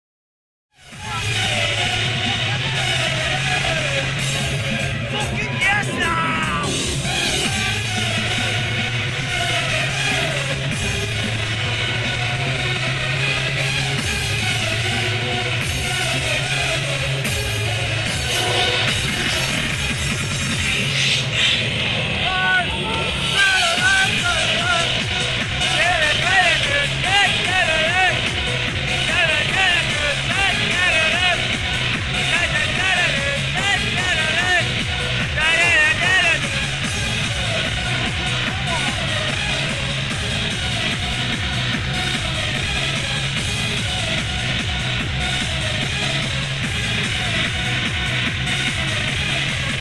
Yoji biomehanika playing hardstyle disco @KIDDSTOCK 2015
Yoji hammering out the classics on a surprisingly sunny English beach